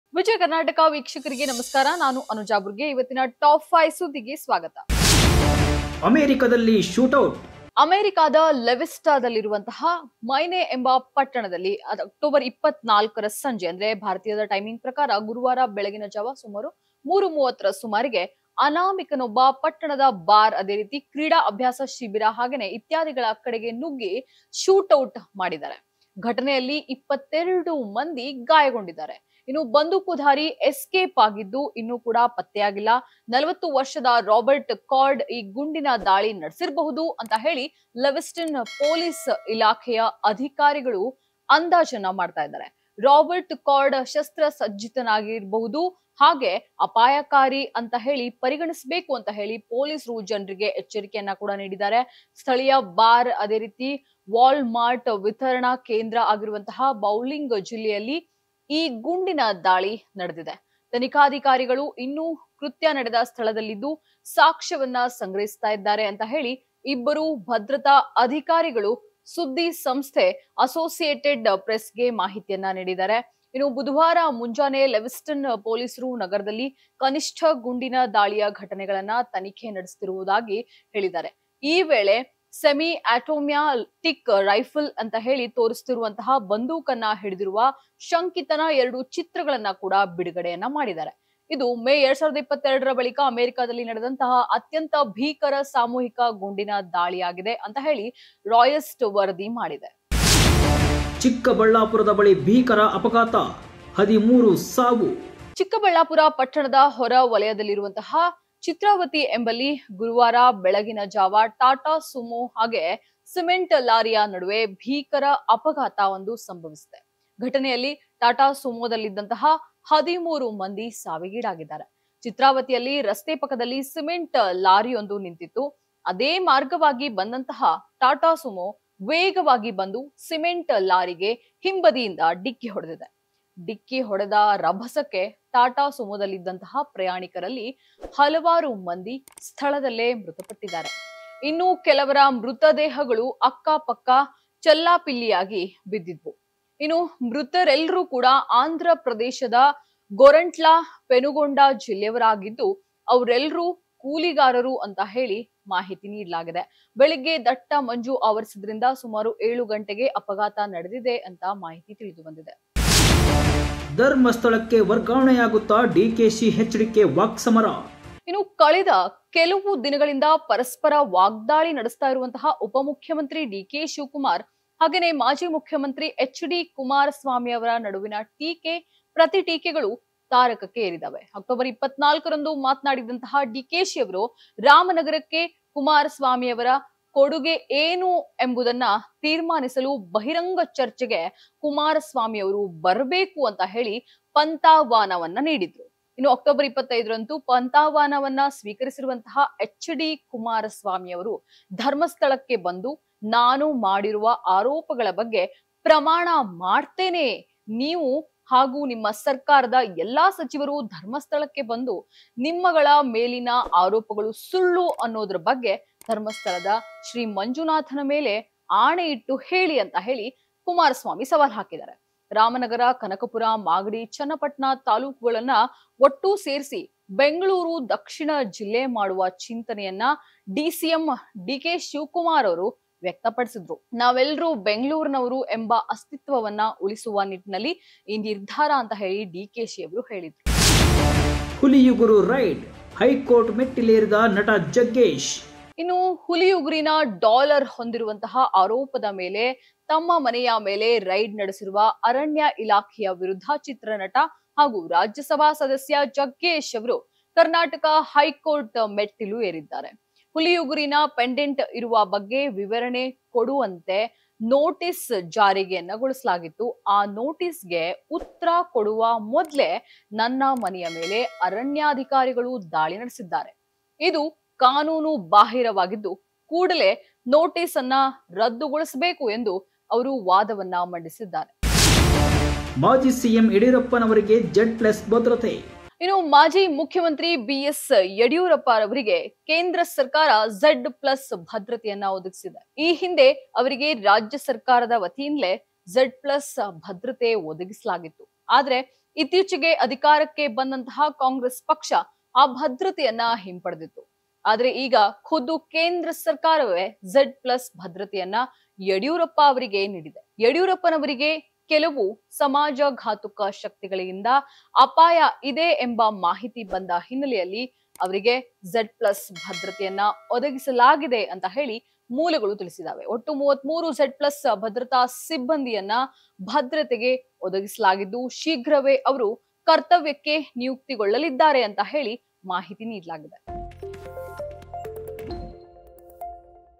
ದಿನದ ಟಾಪ್‌ 5 ಸುದ್ದಿಗಳು | Vijay Karnataka
ಅಮೆರಿಕದ ಲೆವಿಸ್ಟಾದಲ್ಲಿರುವ ಮೈನೆ ಎಂಬ ಪಟ್ಟಣದ ಅ. 24ರ ಸಂಜೆ (ಭಾರತೀಯ ಕಾಲಮಾನದ ಪ್ರಕಾರ ಗುರುವಾರ ಬೆಳಗಿನ ಜಾವ ಸುಮಾರು 3.30ರ ಸುಮಾರಿಗೆ ಅನಾಮಿಕನೊಬ್ಬ ಪಟ್ಟಣದ ಬಾರ್, ಕ್ರೀಡಾ ಅಭ್ಯಾಸ ಶಿಬಿರ ಹಾಗೂ ಇತ್ಯಾದಿಗಳ ಕಡೆಗೆ ನುಗ್ಗಿ ಶೂಟೌಟ್ ನಡೆಸಿದ್ದಾನೆ. ಘಟನೆಯಲ್ಲಿ 22 ಮಂದಿ ಗಾಯಗೊಂಡಿದ್ದಾರೆ. ಬಂದೂಕುಧಾರಿ ಎಸ್ಕೇಪ್ ಆಗಿದ್ದು ಇನ್ನೂ ಪತ್ತೆಯಾಗಿಲ್ಲ. 40 ವರ್ಷದ ರಾಬರ್ಟ್ ಕಾರ್ಡ್ ಈ ಗುಂಡಿನ ದಾಳಿ ನಡೆಸಿರಬಹುದು ಎಂದು ಲೆವಿಸ್ಟನ್ ಪೊಲೀಸ್ ಇಲಾಖೆಯ ಅಧಿಕಾರಿಗಳು ಅಂದಾಜಿಸಿದ್ದಾರೆ. ರಾಬರ್ಟ್‌ ಕಾರ್ಡ್ ಶಸ್ತ್ರಸಜ್ಜಿತನಾಗಿರಬಹುದು ಹಾಗೂ ಅಪಾಯಕಾರಿ ಎಂದು ಪರಿಗಣಿಸಬೇಕು ಎಂದು ಪೊಲೀಸರು ಜನರಿಗೆ ಎಚ್ಚರಿಕೆ ನೀಡಿದ್ದಾರೆ. ಸ್ಥಳೀಯ ಬಾರ್ ಮತ್ತು ವಾಲ್‌ಮಾರ್ಟ್ ವಿತರಣಾ ಕೇಂದ್ರವಾಗಿರುವ ಬೌಲಿಂಗ್ ಅಲ್ಲೆಯಲ್ಲಿ ಈ ಗುಂಡಿನ ದಾಳಿ ನಡೆದಿದೆ. ತನಿಖಾಧಿಕಾರಿಗಳು ಇನ್ನೂ ಕೃತ್ಯ ನಡೆದ ಸ್ಥಳದಲ್ಲಿದ್ದು ಸಾಕ್ಷ್ಯವನ್ನು ಸಂಗ್ರಹಿಸುತ್ತಿದ್ದಾರೆ ಎಂದು ಇಬ್ಬರು ಭದ್ರತಾ ಅಧಿಕಾರಿಗಳು ಸುದ್ದಿ ಸಂಸ್ಥೆ 'ಅಸೋಸಿಯೇಟೆಡ್‌ ಪ್ರೆಸ್‌'ಗೆ ತಿಳಿಸಿದ್ದಾರೆ. ಬುಧವಾರ ಮುಂಜಾನೆ ಲೆವಿಸ್ಟನ್ ಪೊಲೀಸರು ನಗರದಲ್ಲಿ ಕನಿಷ್ಠ ಗುಂಡಿನ ದಾಳಿಯ ಘಟನೆಗಳನ್ನು ತನಿಖೆ ನಡೆಸುತ್ತಿರುವುದಾಗಿ ಹೇಳಿದ್ದರು. ಈ ವೇಳೆ ಸೆಮಿಅಟೋಮ್ಯಾಟಿಕ್ ರೈಫಲ್ ಎಂದು ತೋರುತ್ತಿರುವ ಬಂಧೂಕನ್ನು ಹಿಡಿದಿರುವ ಶಂಕಿತನ ಎರಡು ಚಿತ್ರಗಳನ್ನು ಬಿಡುಗಡೆ ಮಾಡಿದ್ದಾರೆ. ಇದು ಮೇ 2022ರ ಬಳಿಕ ಅಮೆರಿಕದಲ್ಲಿ ನಡೆದ ಅತ್ಯಂತ ಭೀಕರ ಸಾಮೂಹಿಕ ಗುಂಡಿನ ದಾಳಿಯಾಗಿದೆ ಎಂದು 'ರಾಯ್ಟರ್ಸ್‌' ವರದಿ ಮಾಡಿದೆ.